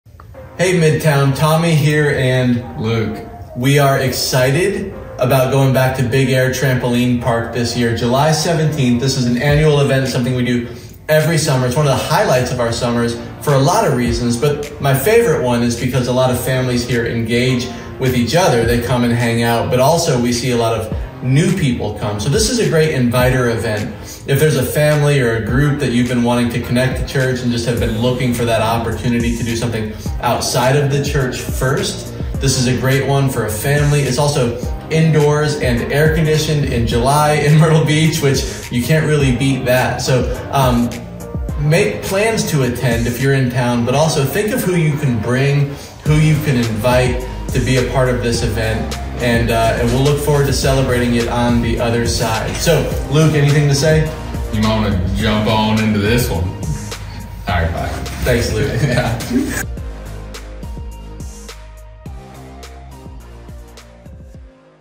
BIg Air 2024
Come join us for a night at the trampoline park! Make plans to join us on July 17th from 6:30-8:30 pm at Big Air Trampoline Park.  This event is for everyone. Anyone who plans to jump needs to register here.  If you do not plan to jump you can just join us on July 19th and hang out with us for the evening.  Attention parents, only register for the jumpers in your group.  All children must be accompanied by an adult.  See you there!